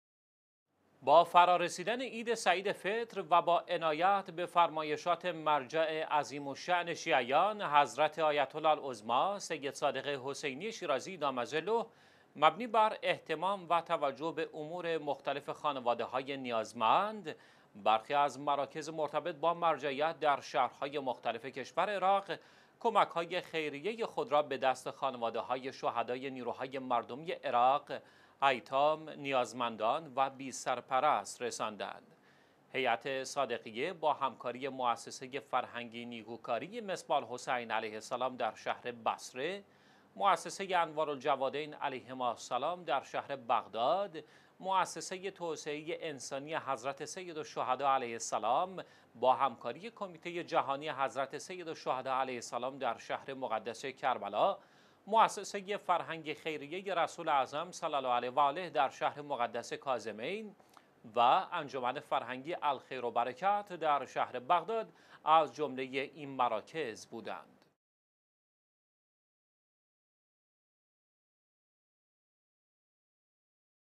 ارائه کمک هاي خيريه به خانواده هاي ايتام و نيازمندان در عيد سعيد فطر
با فرارسيدن عيدفطر و با عنايت به فرمايشات مرجع عظيم الشأن شیعیان، حضرت آيت الله العظمي سيد شيرازي (دام ظله)، مبني بر اهتمام و توجه به امور مختلف خانواده هاي نيازمند؛ برخي از مراکز مرتبط با مرجعيت در شهرهاي مختلف کشور عراق، کمک هاي خيريه خود را به دست خانواده هاي شهداي نيروهاي مردمي عراق، ایتام، نیازمند و بي سرپرست رساندند.

«هيئت صادقيه» با همکاري مؤسسه فرهنگي ـ نيکوکاري «مصباح الحسين عليه السلام» در شهر «بصره»، مؤسسه «أنوار الجوادين عليهما السلام» در شهر «بغداد»، مؤسسه توسعه انساني «حضرت سيدالشهدا عليه السلام» با همکاري «کميته جهاني حضرت سیدالشهداء عليه السلام» در شهر مقدس کربلا، مؤسسه فرهنگي ـ خيريه «رسول اعظم صلي الله عليه وآله» در شهر مقدس کاظمين، و انجمن فرهنگي «الخير والبرکة» در شهر بغداد، از جمله اين مراکز بودند.